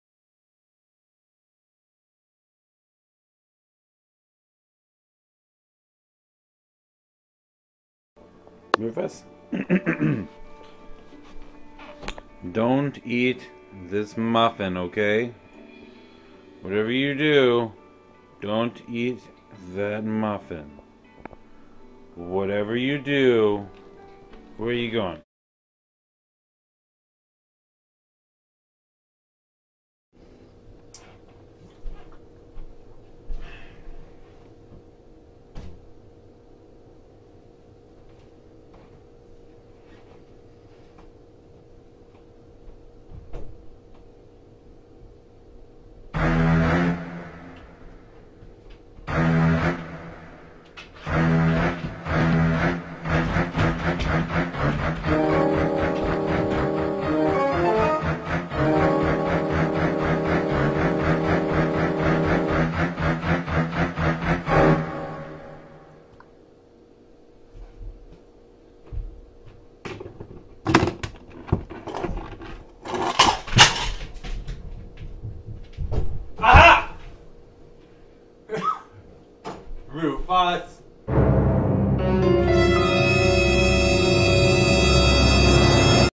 Muffin-stealing dog gets caught in the act!
Rufus, our Old English Sheepdog thinks he's been extra sneaky at stealing muffins and cakes off of the kitchen counter.

We set up an unavoidable trap for him (a muffin tied to a coke can with coins in it) and wait for him to go for the bait. The camera was running the entire time.